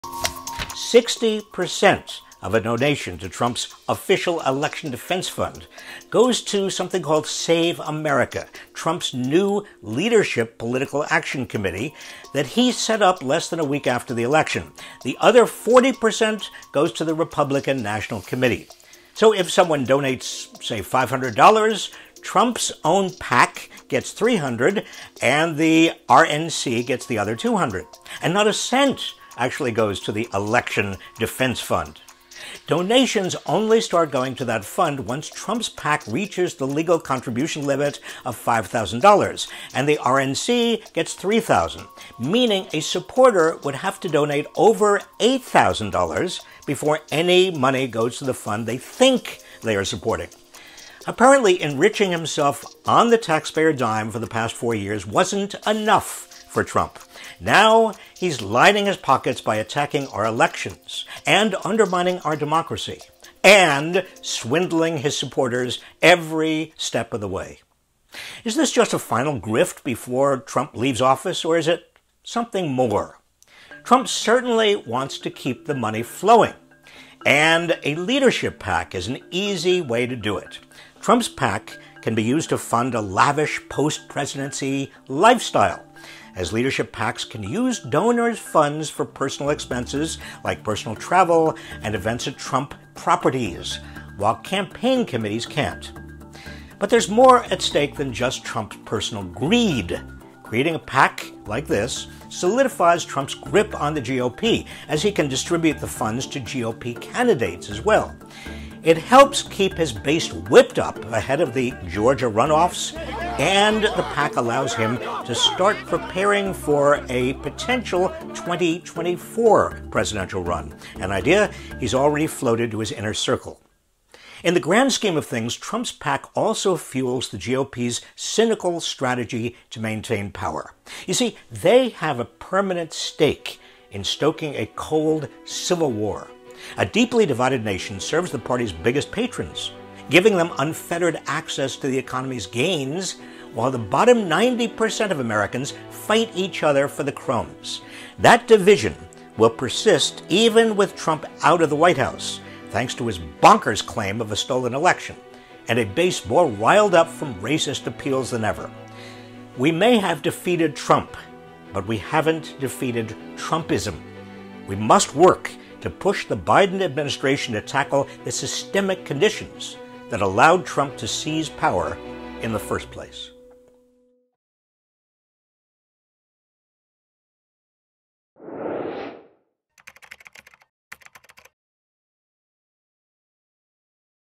How Trump Profited from the Presidency | Robert Reich
Former Secretary of Labor Robert Reich exposes how Trump is swindling his supporters through his meaningless "Official Election Defense Fund".

Trump has launched an “Official Election Defense Fund,” bombarding his supporters with fundraising appeals to supposedly finance the campaign’s ongoing litigation against nonexistent voter fraud.

But the fine print tells a different story.

60 percent of a donation to Trump’s “Official Election Defense Fund” goes to Save America, Trump’s new leadership Political Action Committee that he set up less than a week after the election. The other 40 percent goes to the Republican National Committee. So if someone donates $500, Trump’s PAC gets $300, the RNC gets the other $200, and not a cent actually goes to the election defense fund.

Trump is lining his pockets by attacking our elections and undermining our democracy — and swindling his supporters every step of the way.